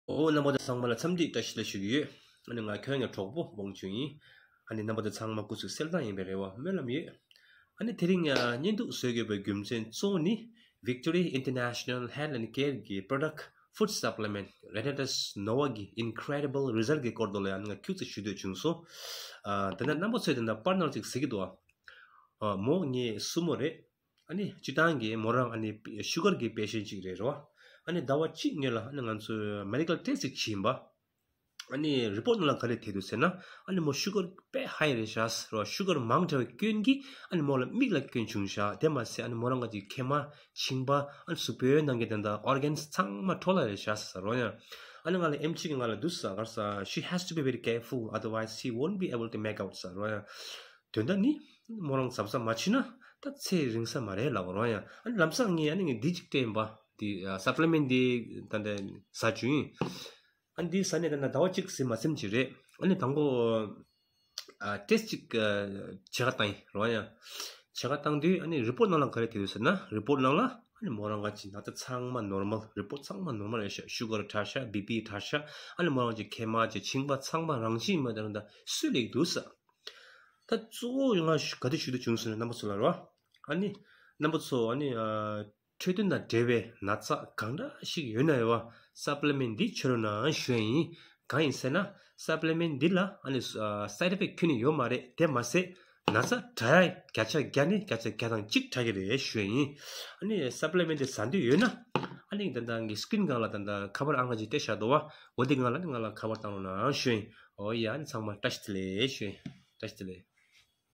August 22, 2024
incredible result of renatus food supplements on high sugar patient